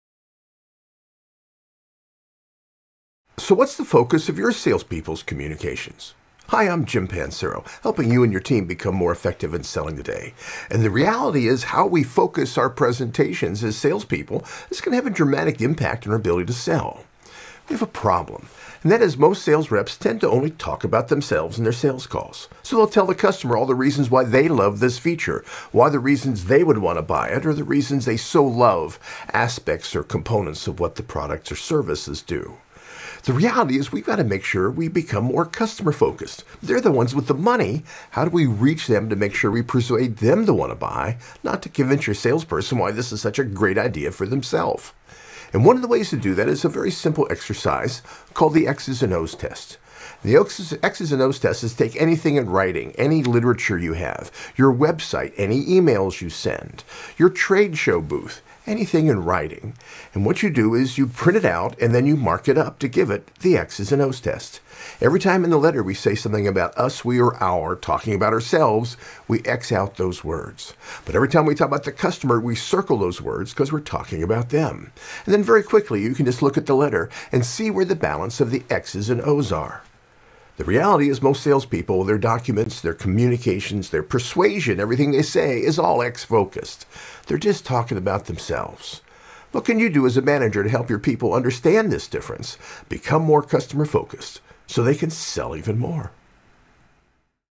Can you pass this simple test - to see if you're really customer focused in your selling language?
How many of the sales messages delivered by your team only talk about themselves, and has little to do with your buyers and their needs? Most reps only sell to one type of buyer…themselves. They’ll tell the buyers why they personally love their product (or service) and why they would want to buy it.

Selling success is based on being more customer-focused. After all, they’re the ones with the money! Salespeople need to persuade buyers by talking about them and the improvements you can bring, not just why you think this is a great product.

One of the easiest ways to be more customer-focused is to use the “X’s & O’s test.” Print out any written document or signage and mark it up. Any time you talk about yourself, your company or products using terms like “us,” we,” or “our,” you “X” out those words…because you’re only talking about your “stuff.” Next circle any words that mention your buyer, their company name or any of their products/services.

Warning! The more you apply this test to anything being sent to a buyer, the more you’ll likely see most of your communications only focuses on you and what you do, instead of focusing on your buyer and what you can do for them.

Check out today’s video where I share more details about this “X’s & O’s” evaluative test you can use that can make all of your communications more customer-focused…and help your team sell even more!